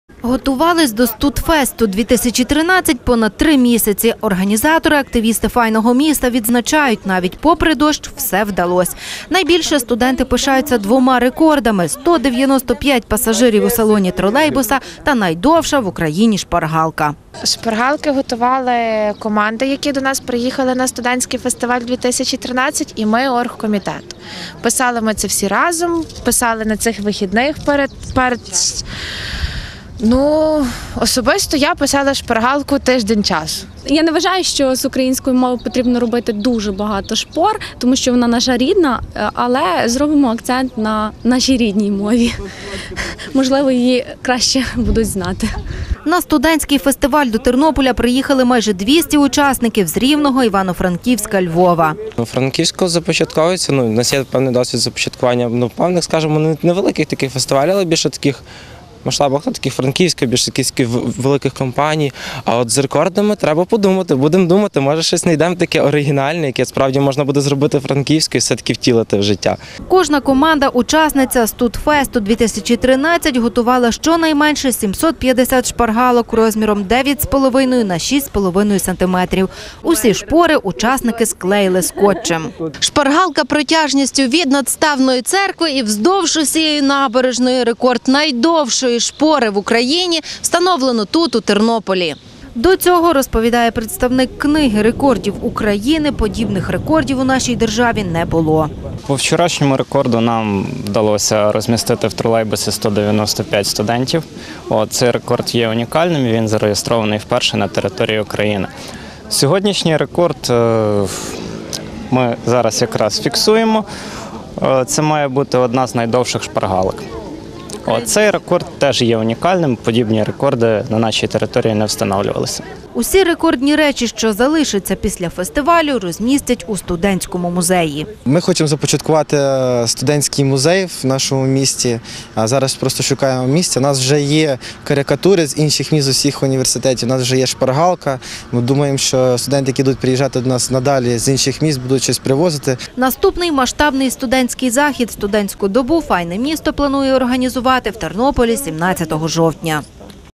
Два рекорди України встановили студенти під час фестивалю «StudFest-2013»
Два рекорди України -- «Найдовша шпаргалка» та «Найбільше студентів у тролейбусі» - встановили цими вихідними у Тернополі. Майже 200 учасників студентського фестивалю «StudFest-2013» з Тернополя, Львова, Рівного та Івано-Франківська змагались за кубок Студентської столиці. Фестиваль відбувався у рамках проекту «Тернопіль -- студентська столиця України» і вперше - у такому масштабі.